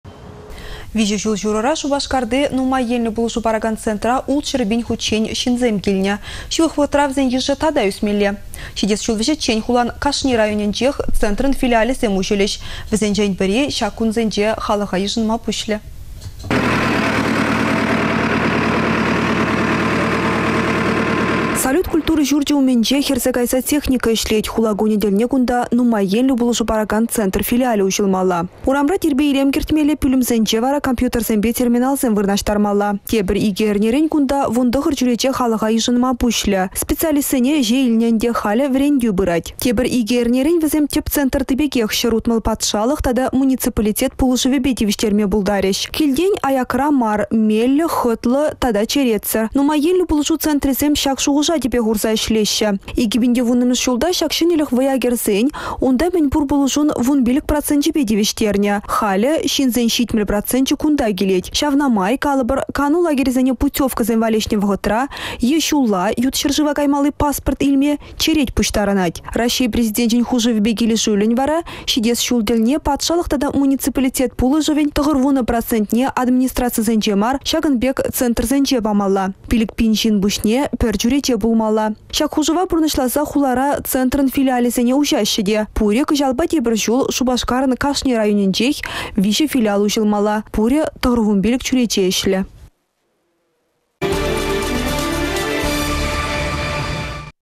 Шупашкарта нумай енлĕ пулăшу паракан центр филиалĕсем уçăлĕç.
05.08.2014
3 çул çурăра Шупашкарти нумай енлĕ пулăшу паракан центра 600 пин хутчен çынсем килнĕ. Çывăх вăхăтра вĕсен йышĕ тата ÿсмелле. Çитес çул вĕçĕччен хулан кашни районĕнчех центрăн филиалĕсем уçăлĕç. Вĕсенчен пĕри çак кунсенче халăха йышăнма пуçлĕ.